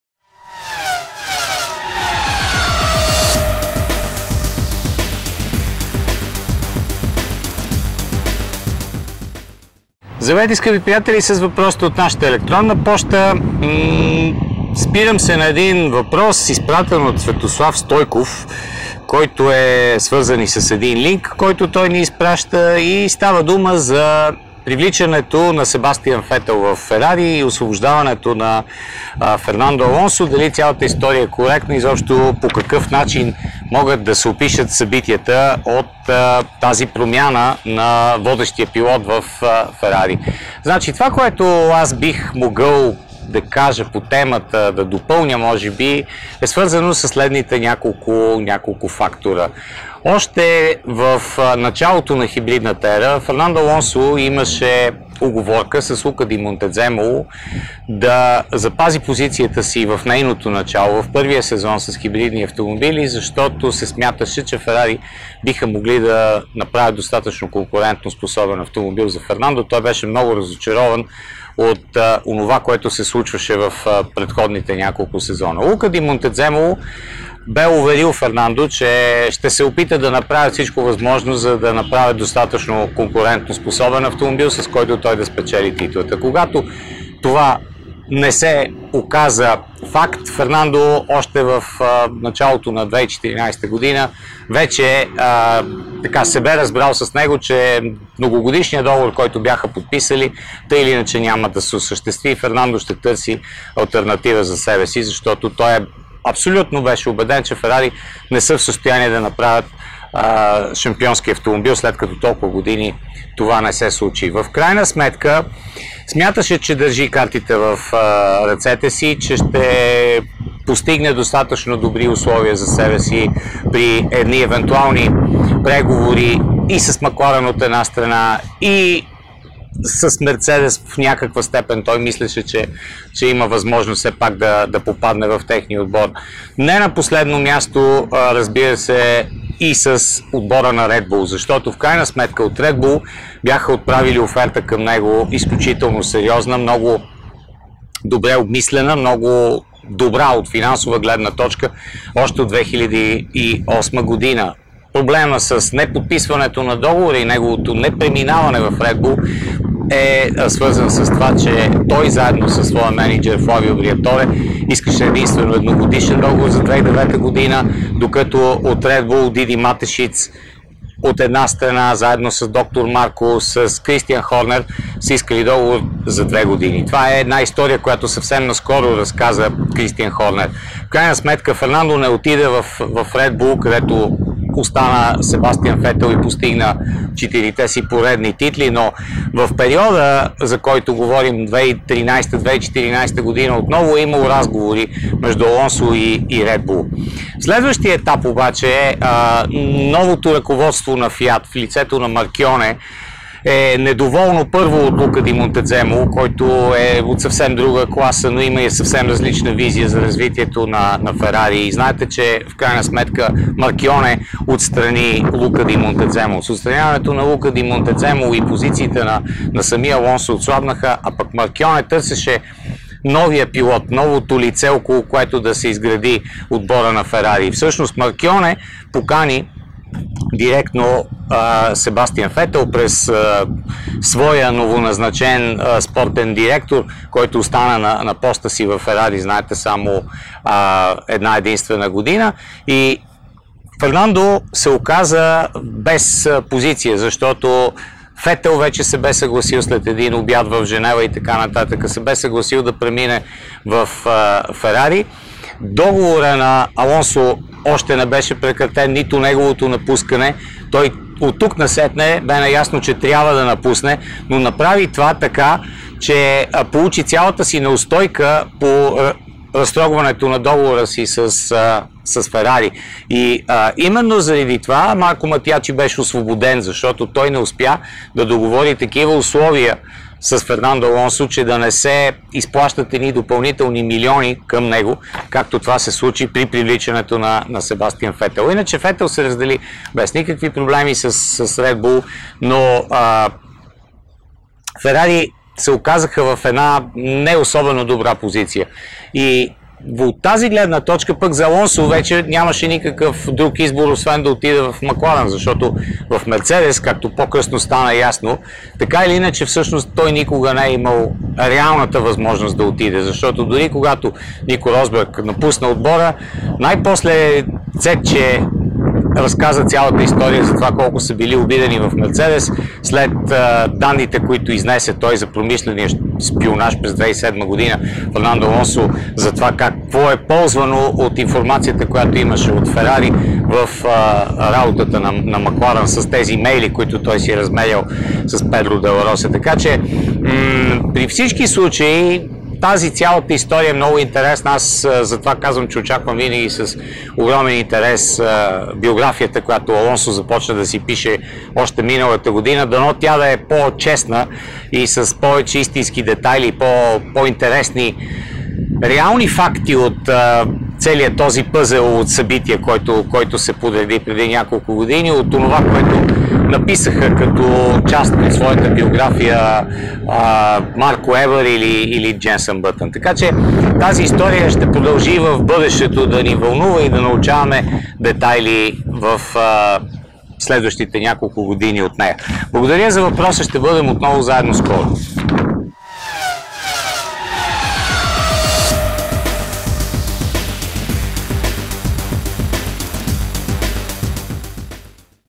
Историята с привличането на Фетел в Ферари и напускането на Алонсо
Попаднах на следната тема която е свързана със привличането на Фетел в Ферари и напускането на Алонсо.

Съмнявам се че всичко е акуратно във статията затова ви моля да направите видео със подробности за цялата история.

Въпрос на Светослав Стойков